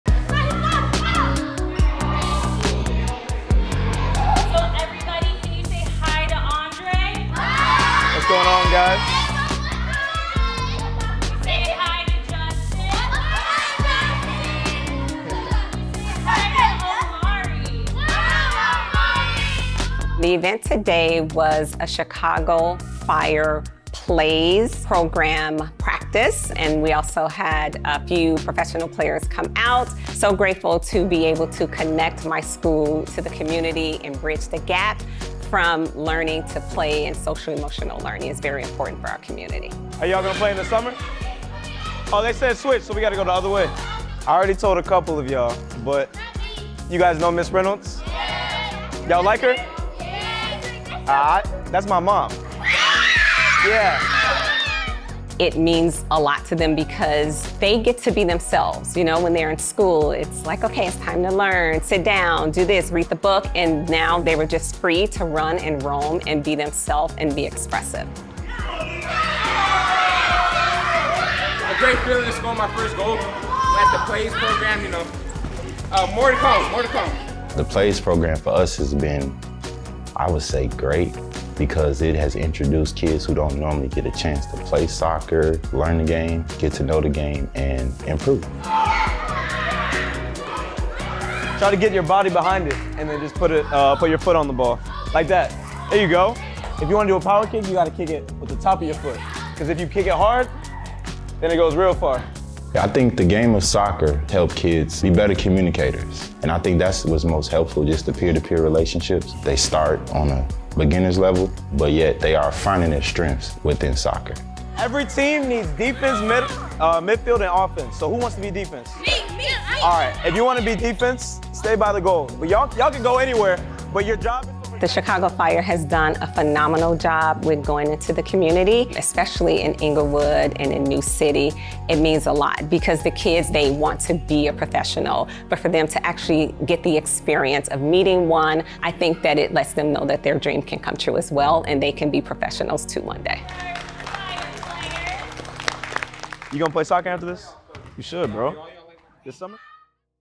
Reynolds brothers visit P.L.A.Y.S. School headed by their principal mom
Andre and Justin Reynolds, both Chicago Fire FC homegrowns, recently took a visit to Dewey School of Excellence, where their mom Charlene is the principal.

Want to get to a game? to view upcoming matches and purchase tickets!